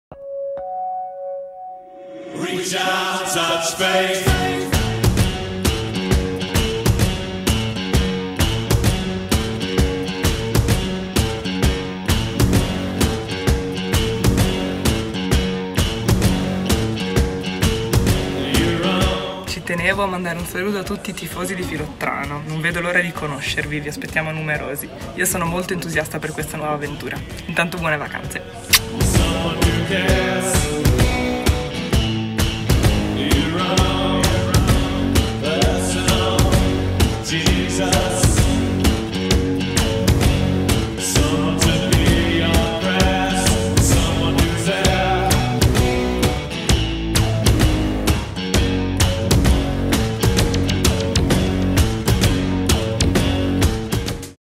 Lardini Filottrano 2019-2020: GAIA MORETTO
Scopriamo le protagoniste della Lardini Filottrano per la stagione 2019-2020 della Serie A1 Samsung Volley Cup!
La Campionessa d'Italia Gaia Moretto al centro!